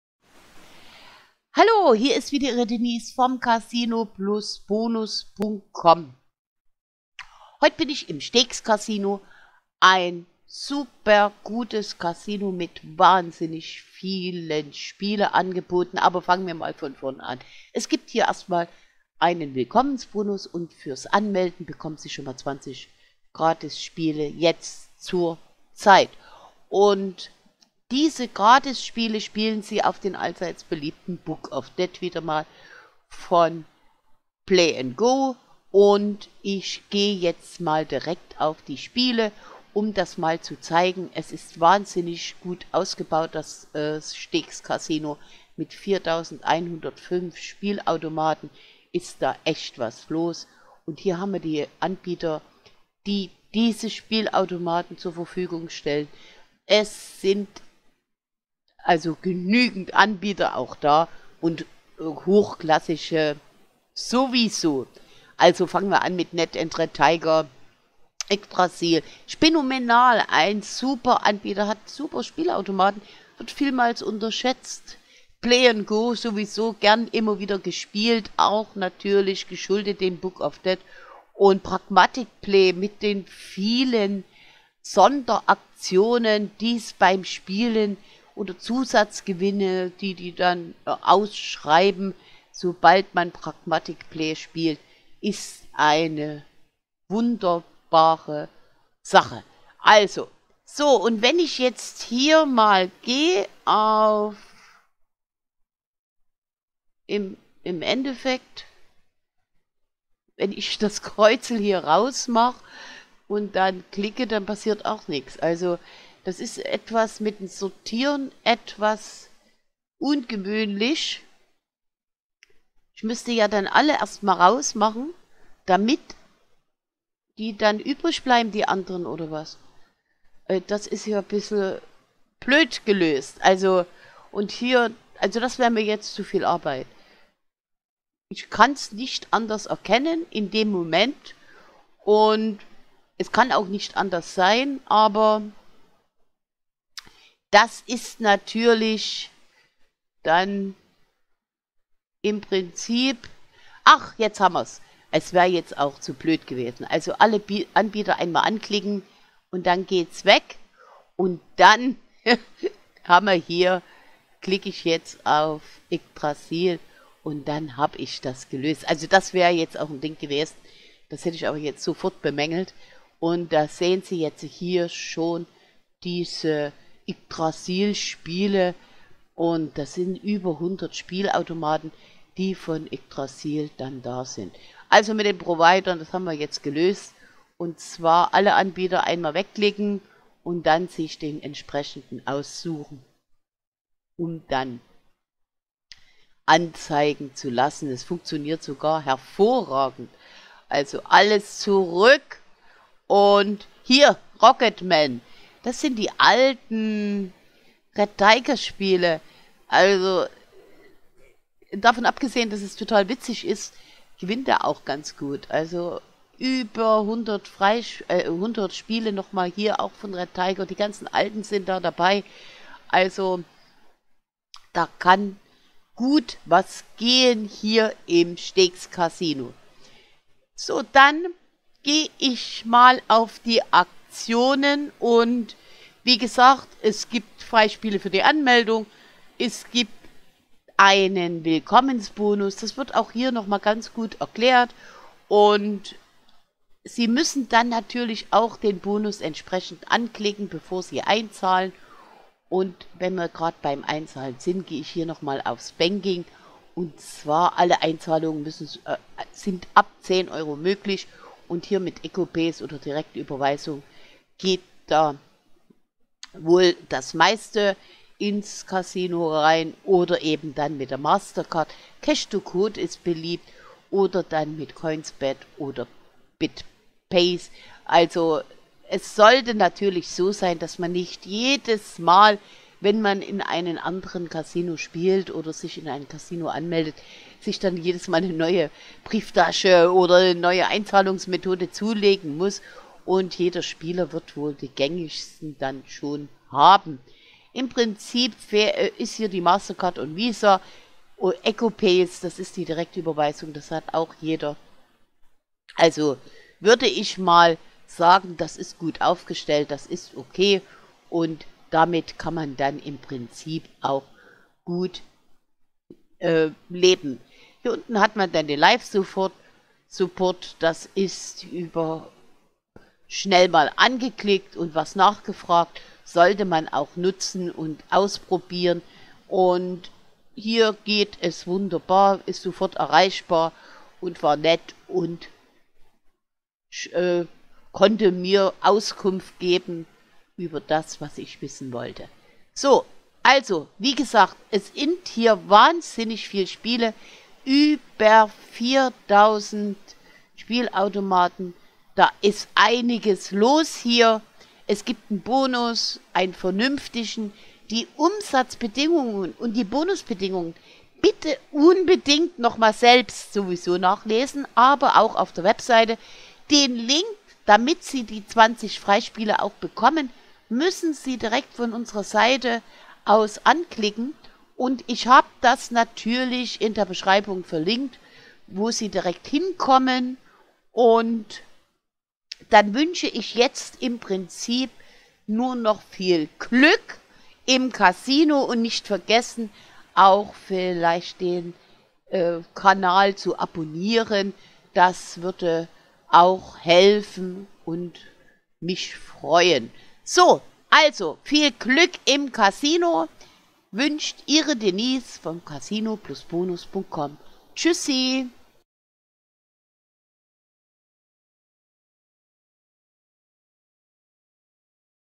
Stakes Casino Test
Mehr Informationen zum Stakes Casino und den kostenlosen Freispielen oder auch Bonusinfos bekommen Sie

+ 20 Stakes Casino Freispiele ohne Einzahlung
–/+ 100 % Bonus bis zu 250 PLUS 100 Freispiele auf die erste Einzahlung (10 Stück am Tag)
–/+ viele weitere Bonusangebote!
+ Ausgeglichenes Verhältnis von mind. Einzahlung von 10 € und min. Auszahlung von 20 €
– Live Spiele, Video Poker und auch Tischspiele werden überhaupt nicht an die Umsatzbedingungen angerechnet
+ Guter Software Mix mit über 4000 Spielautomaten
+ Auszahlung nach 0-2 Tagen Wartezeit sofort mit E-Wallets.
–/+  Auszahlungslimit zu niedrig  für Highroller – mit 2000 € täglich und 5,000 € in der Woche